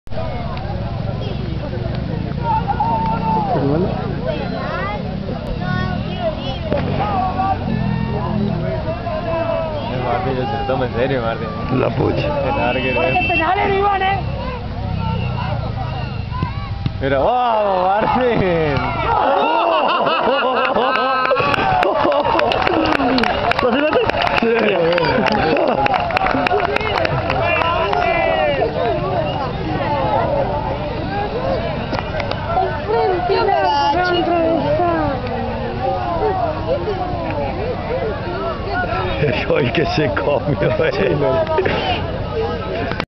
PARTIDO A BENEFICIO ARROYITO CORDOBA, OBOLO, CHINO LUNA, PEREZ GARCIA Y OTROS
PARTIDO A BENEFICIO PENAL ATAJADO A MAURO IVAN OBOLO Y EL REBOTE SE LO RE COME UNO JOJOJOJO ARROYITO CORDOBA